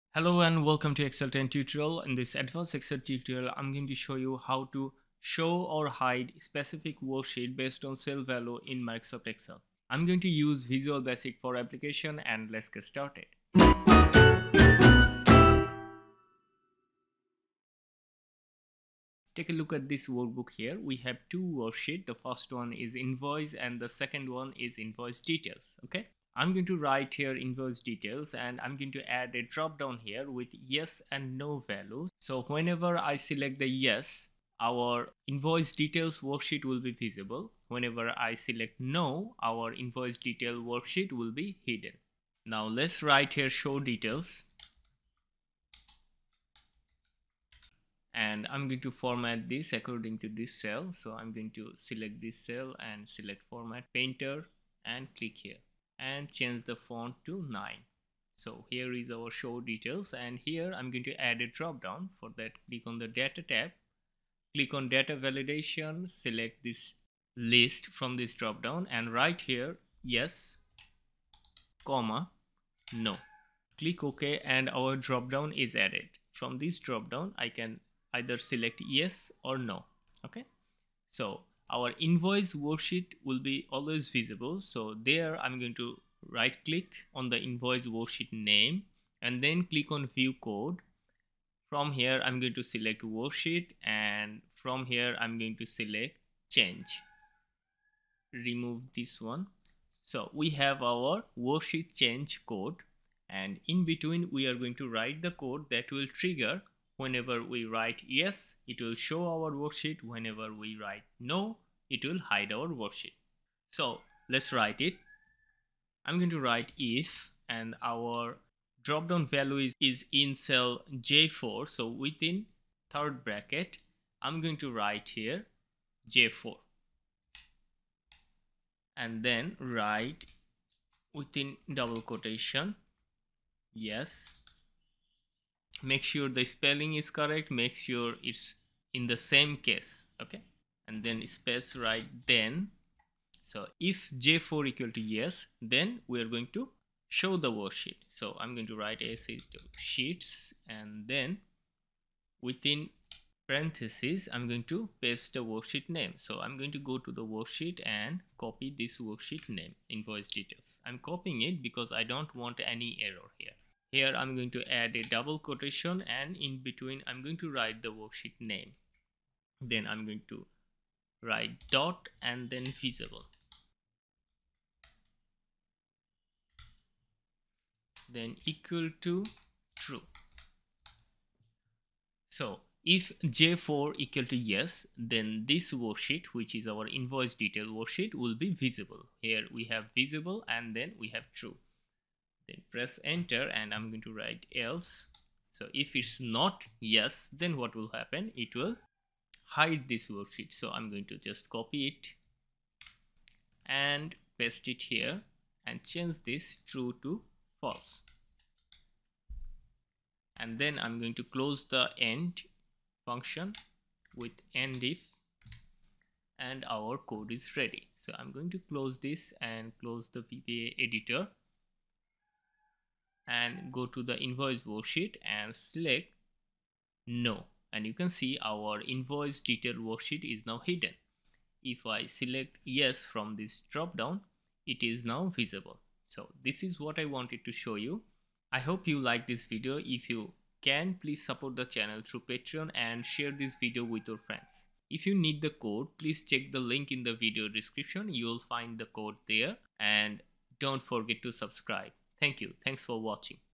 How to Hide or Unhide a Specific Worksheet Based on Cell Value in Another Sheet In Excel
*Showing or Hiding Worksheets Based on Specific Text in Microsoft Excel Using VBA*
Microsoft Excel provides a powerful scripting language called VBA (Visual Basic for Applications) that allows users to automate tasks and enhance the functionality of their spreadsheets. One common requirement is the ability to show or hide worksheets based on specific criteria. In this advance excel tutorial, we'll explore how to achieve this using VBA code.

*Understanding the VBA Code*
The code is placed within a specific worksheet, and it is triggered whenever there is a change on that particular sheet. The code checks if the value in cell J4 is equal to "Yes." If it is, the worksheet named "Invoice details" is set to be visible; otherwise, it is set to be hidden.

*Step-by-Step Guide:*
1. Open Excel and Access the Visual Basic for Applications (VBA) Editor
- Right-click on the main worksheet name and select “View code”.

2. Insert the VBA Code
- Copy and paste the provided VBA code into the module.

3. Adjust the Code for Your Worksheet
- Modify the code according to your specific requirements. In the provided code, the condition checks if the value in cell J4 is "Yes." You can change this to any other cell or criteria based on your needs.

4. Test the Code
- Close the VBA editor and return to your Excel workbook.
- Change the value in the specified cell (in this case, J4) to see the worksheet visibility change accordingly.
- In our video we created a dropdown list to change the value of J4

*Additional Tips:*
a. Case Sensitivity
Keep in mind that VBA is case-sensitive. "Yes" is not the same as "yes." Ensure that your code matches the case of the text in the cell.
By following these steps and customizing the code to fit your specific needs, you can efficiently show or hide worksheets in Microsoft Excel based on the content of a particular cell using VBA.